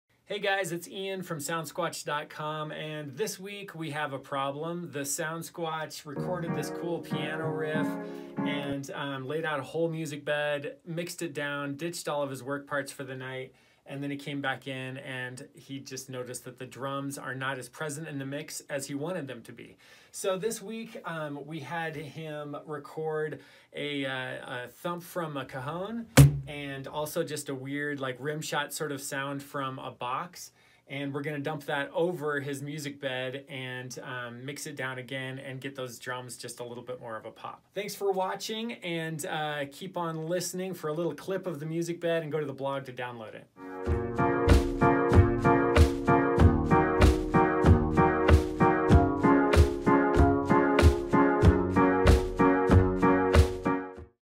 Fixing the Soundsquatch's Music Bed
This week the Soundsquatch was working on a music bed  to use in a video and mixed it down.  He came in the next day to discover that the drums weren't present enough in the mix for his liking.

Here's how we fixed it...

1.  We had the SoundSquatch record  a Cajon kick drum (which was a little high, so we stretched to a bit to lower the pitch.

2.  We had the Soundsquatch record a rim shot sort of sound by hitting a box.

3.  We layered a clap over that rim-shot sound to give it even more pop.

Then we layered it over each beat of the song by hand, and remixed it.